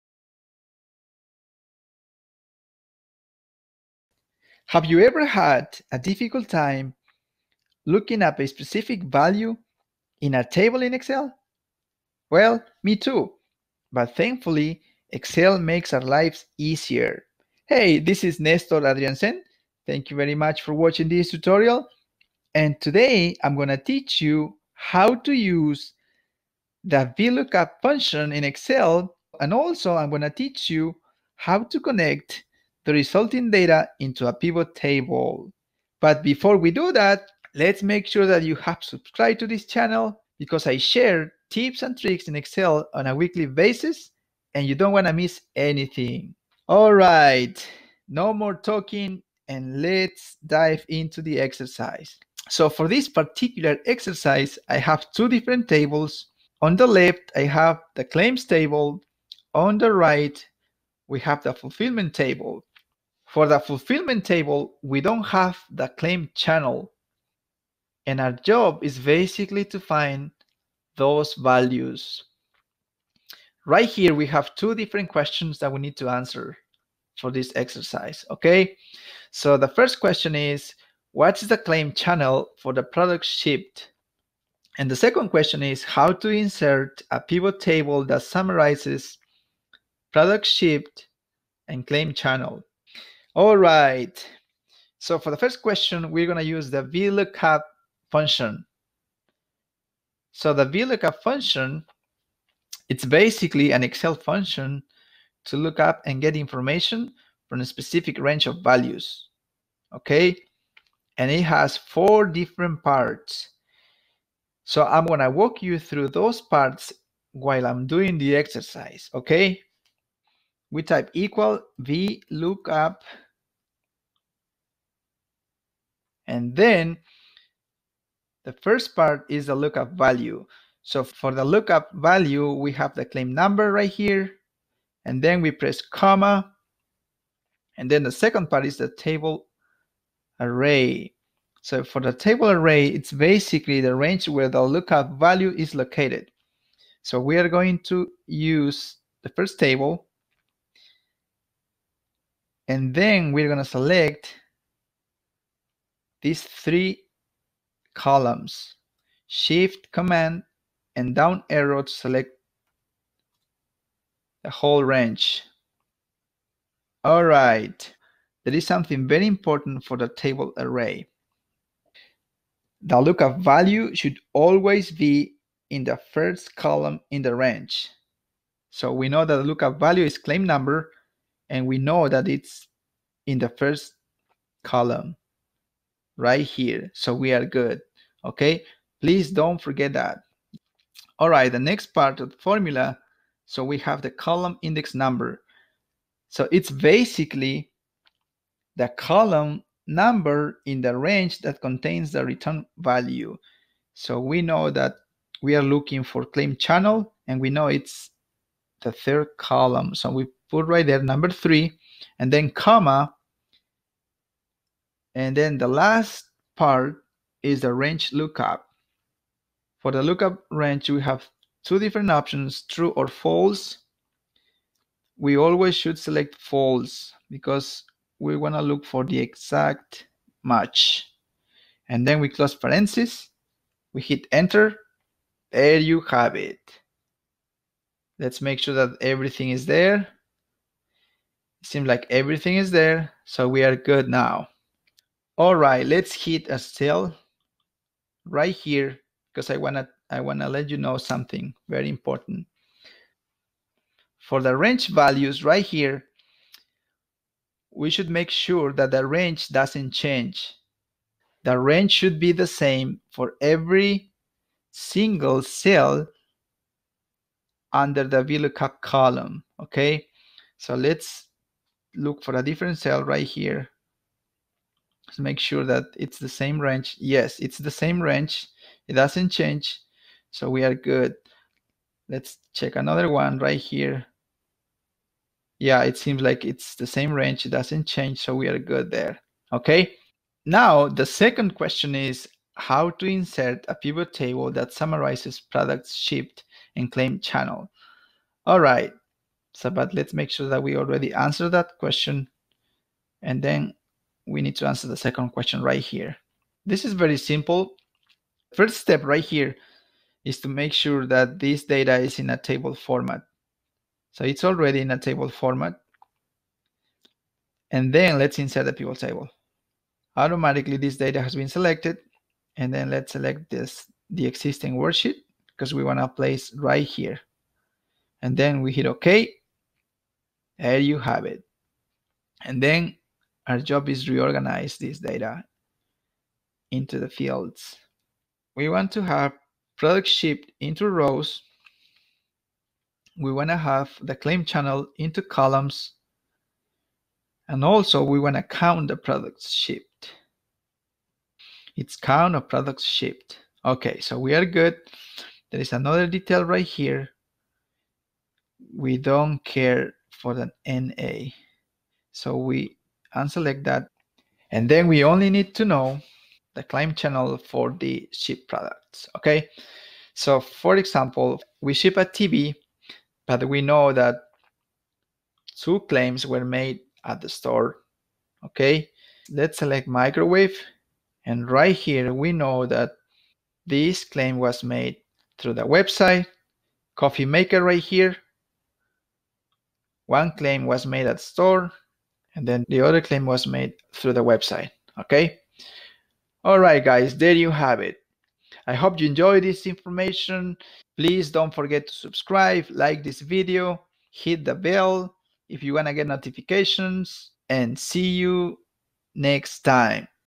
Vlookup and Pivot Table in Excel 2016: Mac
Vlookup & Pivot Table in a Mac:

We will be using the Vlookup function to match values from two different tables, then we will be connecting these values into a Pivot Table. In addition, there are a couple of Excel tips in the video as well.